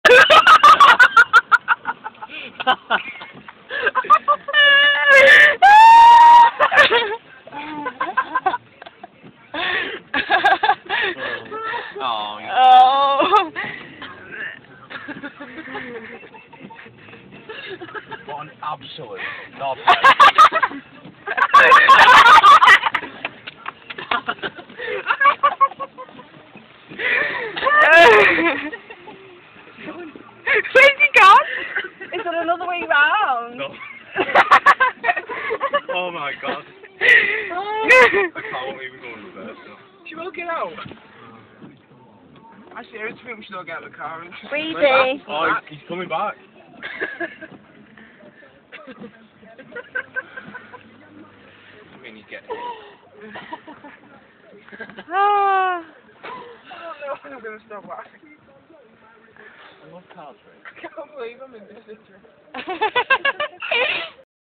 Blocking Jack in
Blocking the road so Jack can't get past